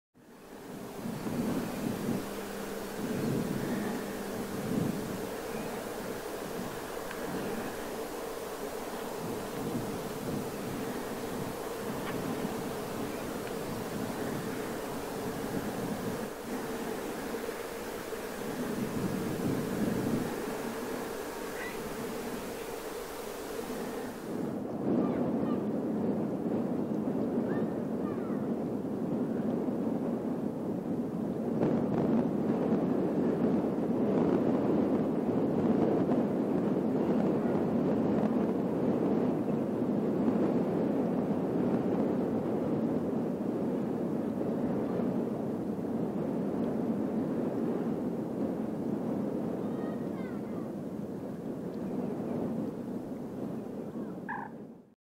IBIS Sacré/Threskiornis aethiopicus/Automne ! BRUITX
Observations et Enregistrements dans le Golfe du Morbihan : Octobre 2022 !